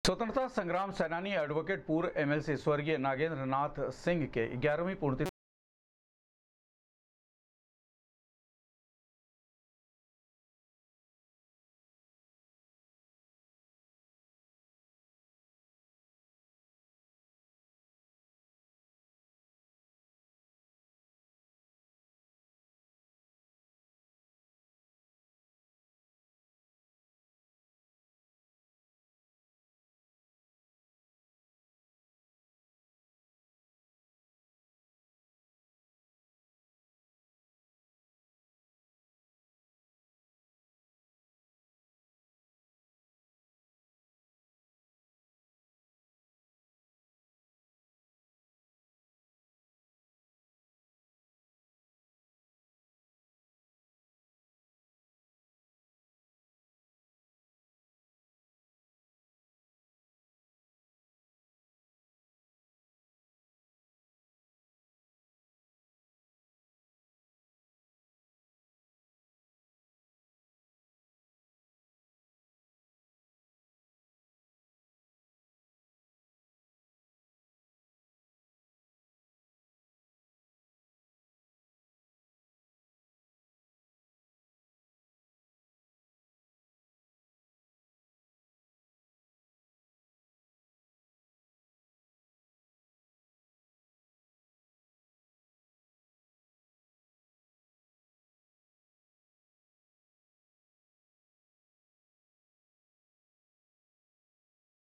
स्वतंत्रता संग्राम सेनानी,एडवोकेट व पूर्व एमएलसी स्वर्गीय नागेंद्र नाथ सिंह के ग्यारहवीं पुण्यतिथि
स्वतंत्रता संग्राम सेनानी,एडवोकेट व पूर्व एमएलसी स्वर्गीय नागेंद्र नाथ सिंह के ग्यारहवीं पुण्यतिथि के अवसर पर श्रद्धांजलि सभा का किया गया आयोजन,मुख्य अतिथि हैदराबाद के अर्थशास्त्री व राजनीतिक विश्लेषक परकाला प्रभाकर सहित अन्य लोगों ने उनके चित्रों पर पुष्पांजलि अर्पित कर दी श्रद्धांजलि, जनपद में उत्कृष्ट कार्य करने वाले विभूतियो को किया गया सम्मानित